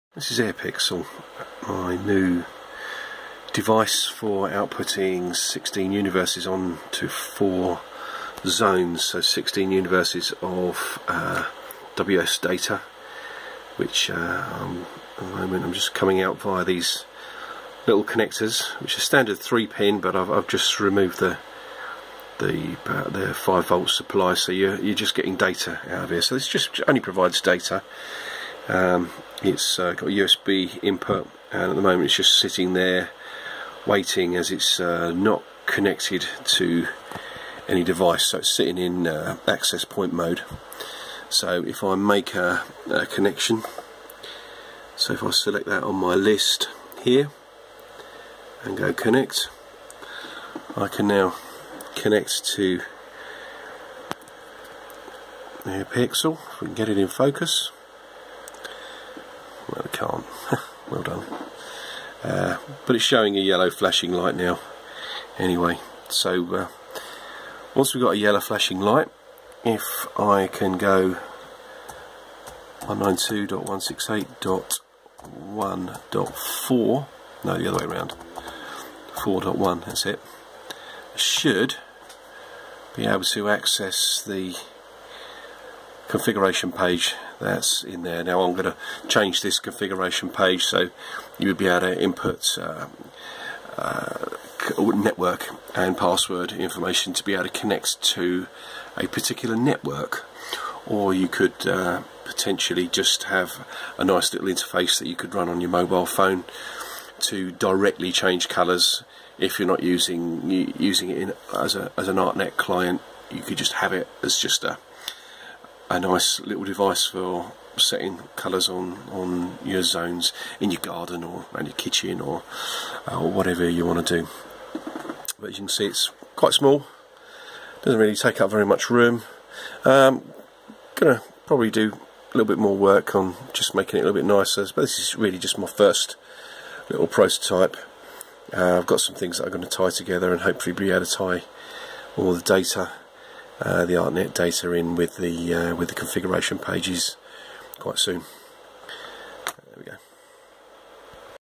SmartShow airPixel : 16 Universe Wireless Artnet sACN DMX to WS2812 Neopixel Driver
16 Universes of Artnet/sACN (E1.31) to 4 zones x 4 universes of WS2812/WS2811/SK6812 (NRZ) data for Pixel LEDs. Configuration via Browser